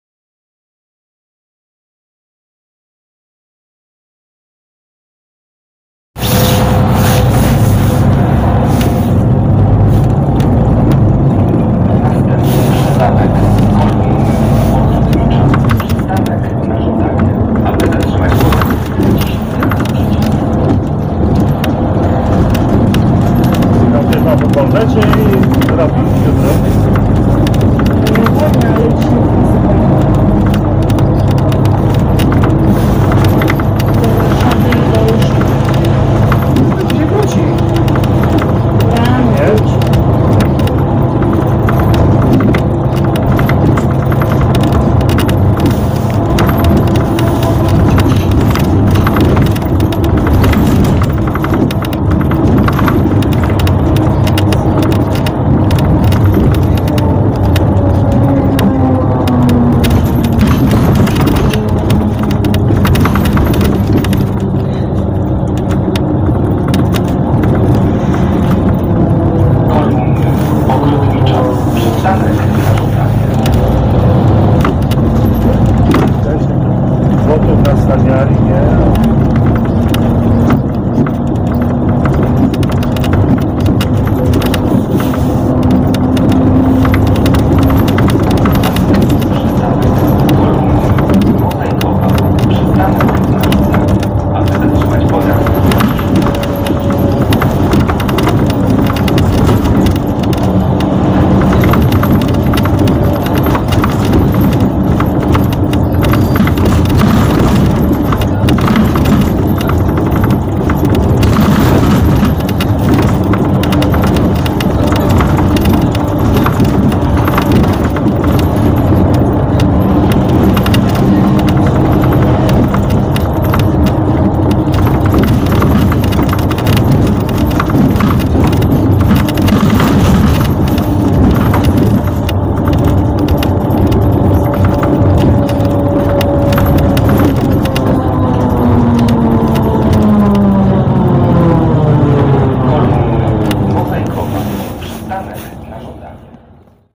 [*] Mercedes Benz Conecto LF E6 #2460 (Linia 92A 🚍) MPK Łódź [*]
Zdjęcie w miniaturce filmu pochodzi z Buslodza.
Autor zdjęcia: Marcin Wnukowicz

Linia 92A [PL.Niepodległości - Stróża Kolumny]

Dane Techniczne:
Model Autobusu: Mercedes Benz Conecto LF E6 #2460 [*]
Rok Wycofania: 2023 (Pojazd został zakupiony przez SimKol Katowice i posiada numer #108).
Silnik:
Mercedes Benz OM936 (300 KM)
Skrzynia Biegów:
ZF 6AP1200 B EcoLife (6 Biegów)
Zajezdnia: Nowe Sady [EA-2]

Przystanki:
Kolumny-Mahoniowa NŻ
Kolumny-Ogrodnicza NŻ
Kolumny-Mozaikowa NŻ